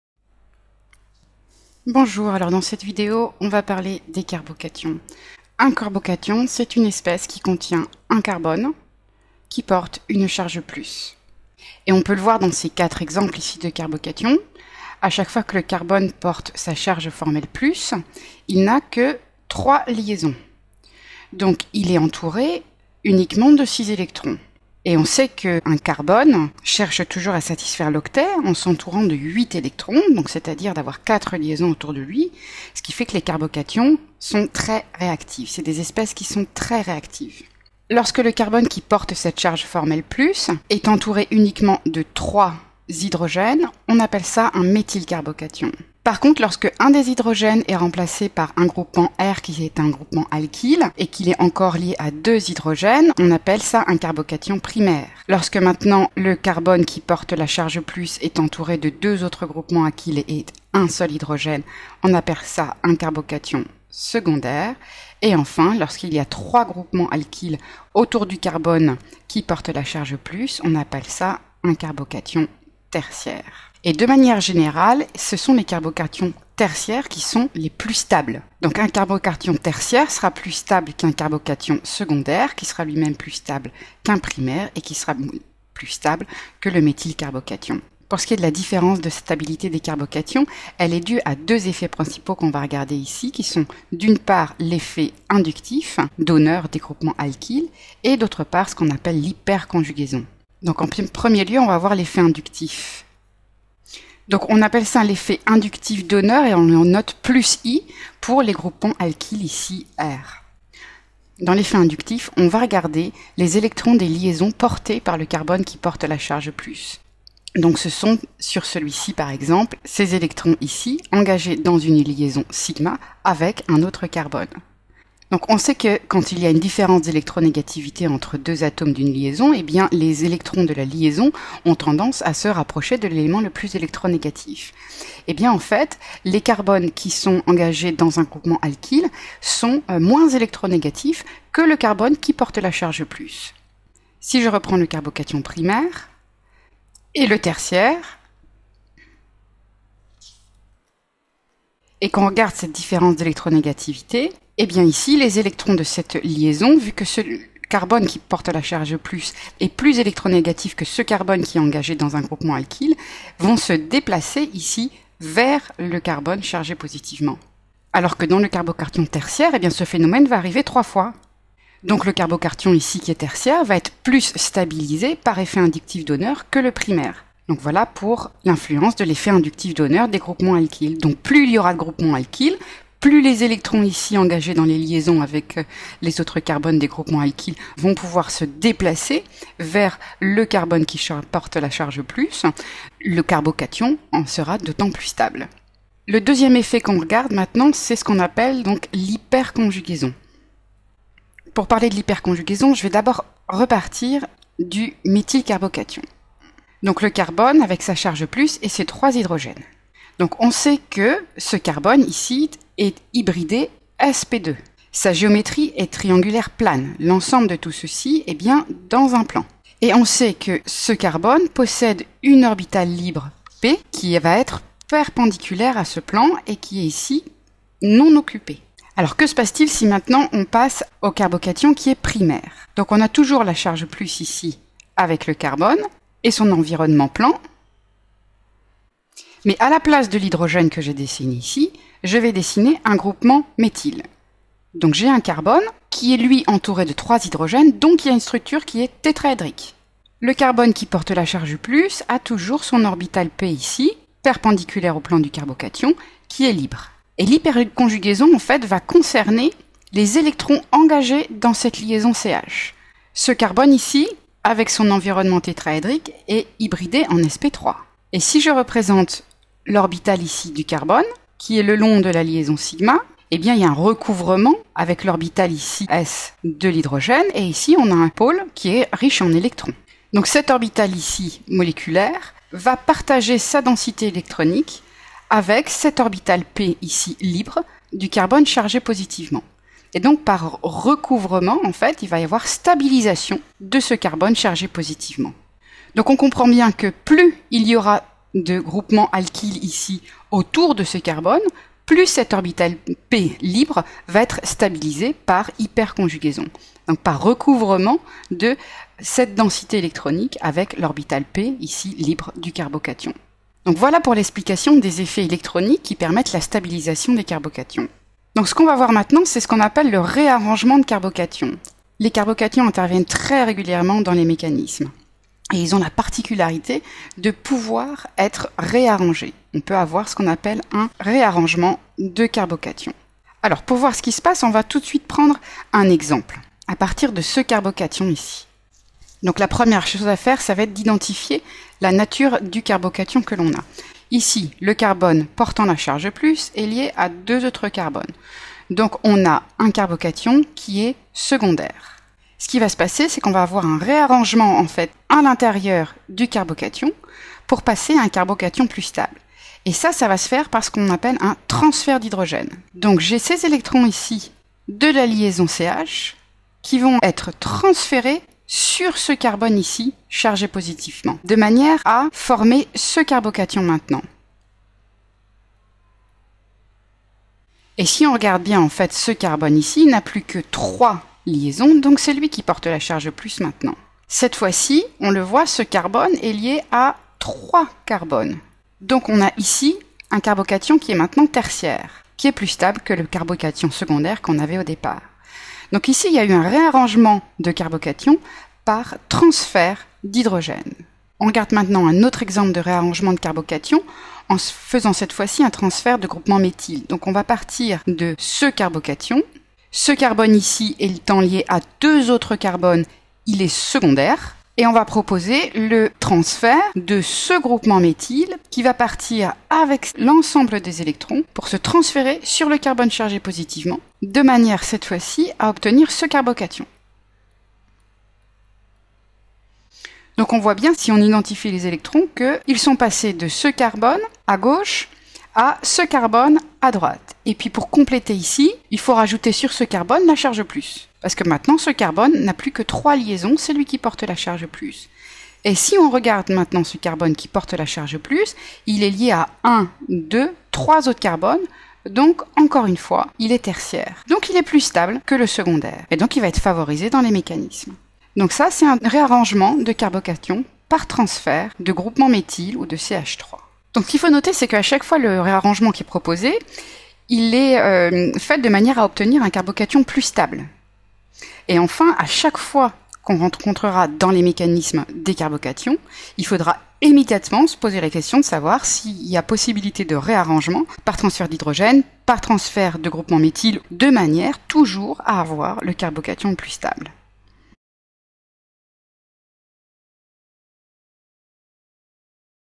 Carbocations : Stabilité et Réarrangement
Comparaison de la stabilité des carbocations et hyperconjugaison. Mécanisme de réarrangement de carbocation incluant la migration d'un hydrure ou d'un groupe méthyle.